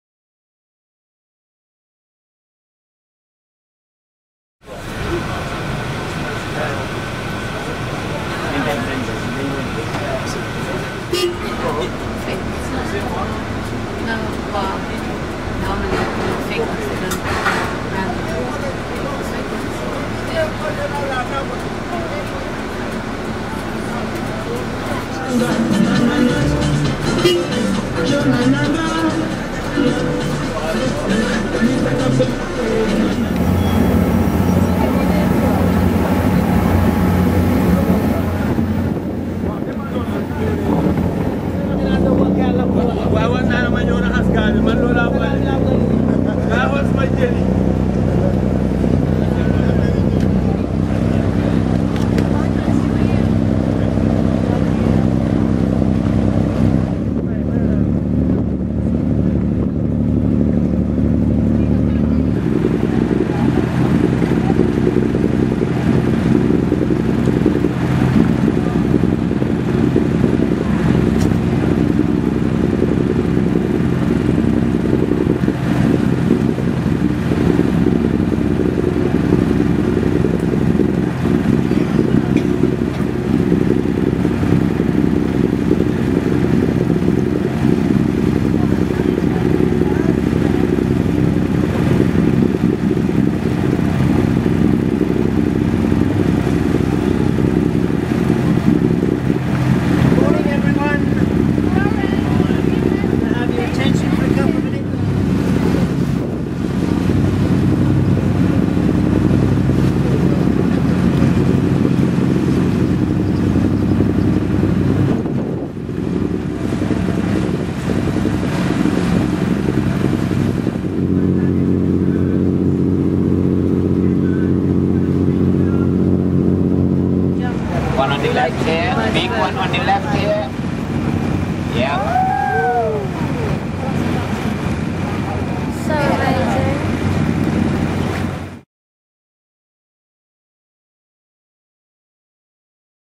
Banjul Port and Barra Ferry Terminal, The Gambia - 4th March, 2010
Views of Banjul port and Barra Ferry Terminal including the quayside, fishing boat with hanging yellow fin tuna, container ships, oil tankers and dolphins jumping in the water.